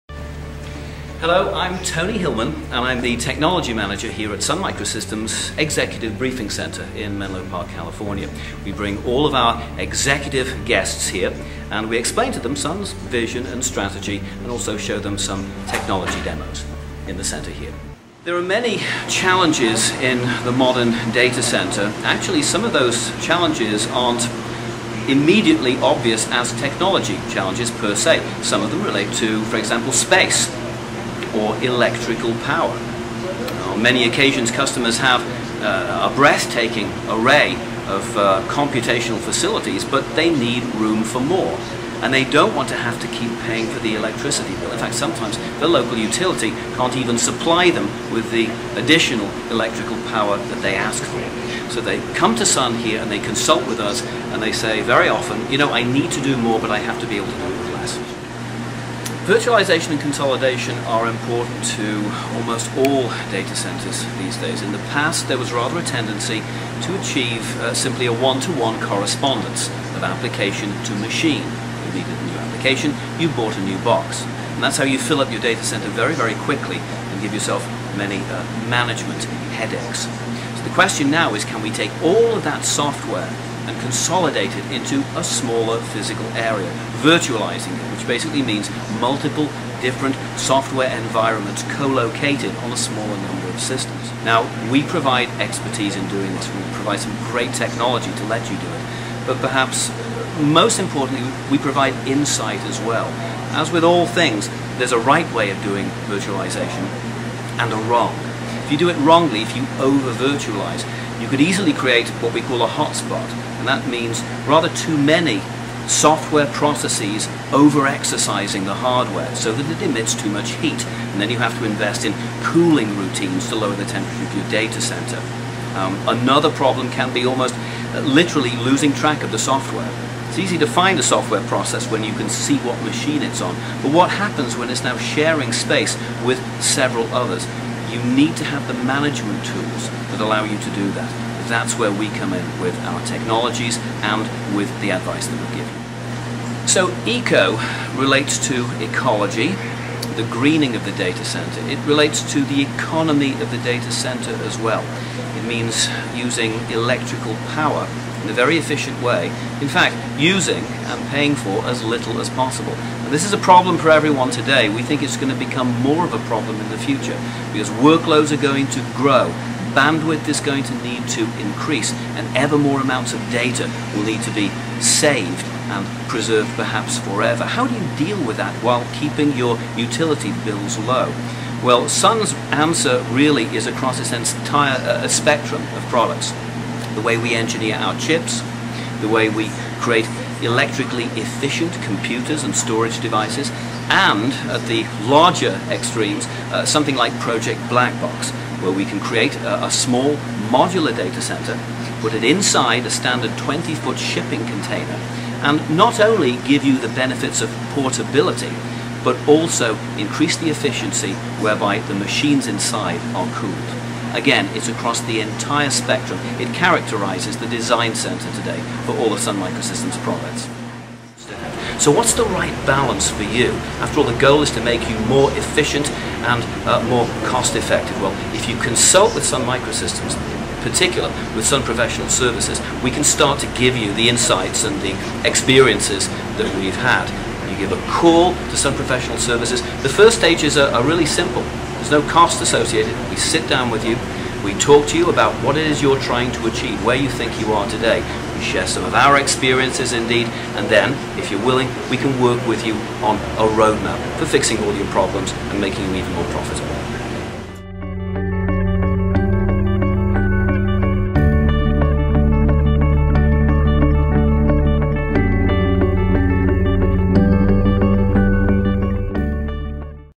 The Modern Datacenter with Sun Professional Services
Take a trip through Sun's Executive Briefing Center to learn about common datacenter challenges, virtualization and consolidation and taking an eco-friendly approach.

Sun Professional Services brings the insight, technology and experience to cost-effectively and efficiently architect the modern datacenters of today.